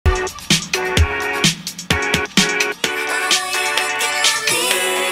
Tough squirrel
Squirrel tries to act tough.  But, ends up just being cute instead.  There are so many animals right now.  It's baby mania too.